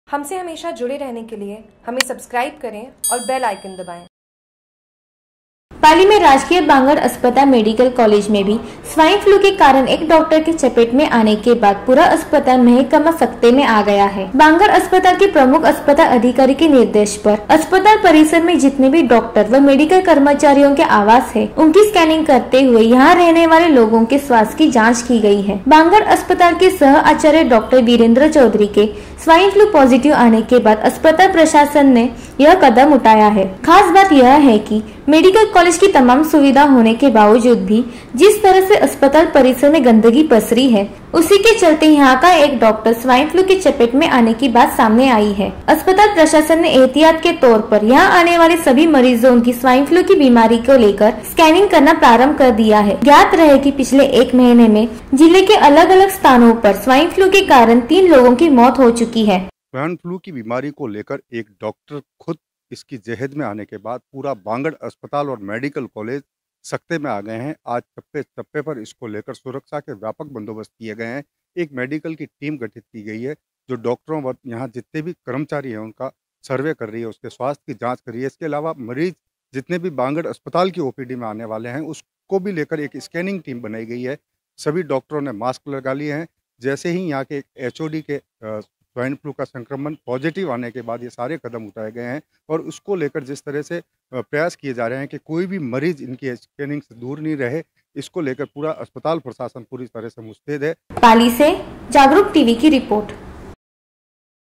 JAGRUK TV : पाली : स्वाइन फ्लू के चपेट में आया डॉक्टर
चिकित्सालय मेडिकल कॉलेज में भी स्वाइन फ्लू के कारण  एक चिकित्सक के चपेट में आने के बाद पूरा चिकित्सा महकमा सकते में आ गया है ।
      बांगड़ अस्पताल के प्रमुख चिकित्सा अधिकारी के निर्देश पर अस्पताल परिसर में जितने भी डॉक्टर व मेडिकल कर्मचारियों के आवास हैं उनकी स्कैनिंग कराते हुए या रहने वाले लोगों के स्वास्थ्य की जांच की गई है बांगड़ अस्पताल के सह आचार्य डॉक्टर वीरेंद्र चौधरी के स्वाइन फ्लू पॉजिटिव आने के बाद अस्पताल प्रसाद प्रशासन ने यह कदम उठाया है खास बात यह है कि मेडिकल कॉलेज की तमाम सुविधाएं होने के बावजूद भी जिस तरह से अस्पताल परिसर में गंदगी पसरी है